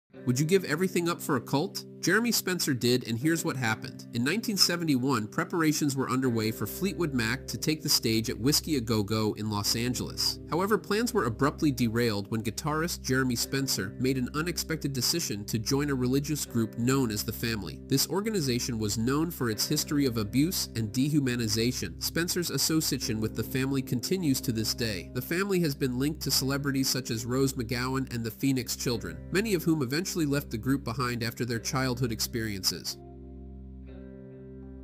The INSANE Cult Story Of Fleetwood Mac Member!! 😯
Would you give everything up for a cult? Jeremy Spencer did and here’s what happened.
In 1971, preparations were underway for Fleetwood Mac to take the stage at Whiskey a Go Go in Los Angeles. However, plans were abruptly derailed when guitarist Jeremy Spencer made an unexpected decision to join a religious group known as The Family. This organization was known for its history of abuse and dehumanization. Spencer's association with The Family continues to this day. The Family has been linked to celebrities such as Rose McGowan and the Phoenix children, many of whom eventually left the group behind after their childhood experiences.